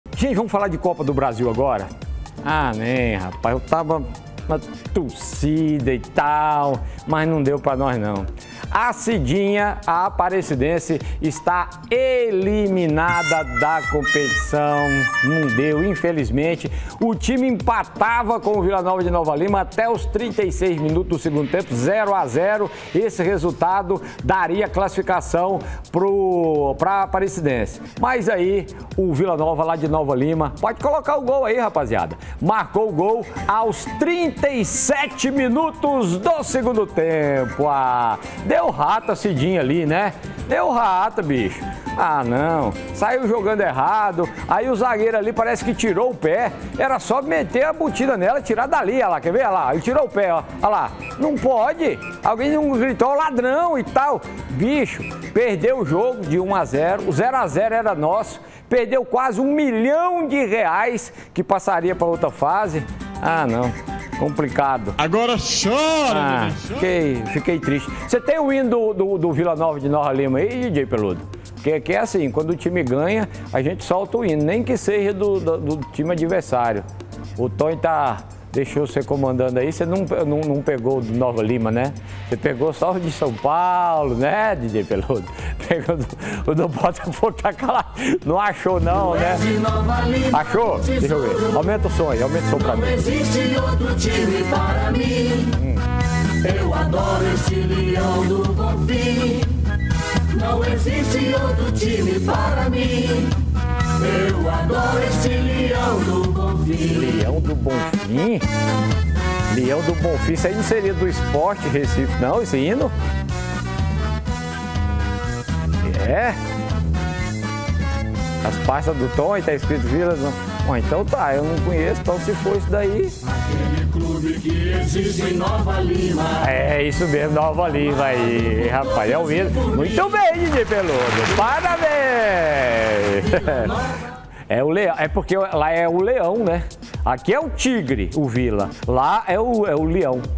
APARECIDENSE PERDE PRO VILLA NOVA-MG E ESTÁ FORA
COPA DO BRASIL. A CIDINHA ESTÁ ELIMINADA DA COMPETIÇÃO. O TIME PERDEU PARA O VILLA NOVA DE NOVA LIMA.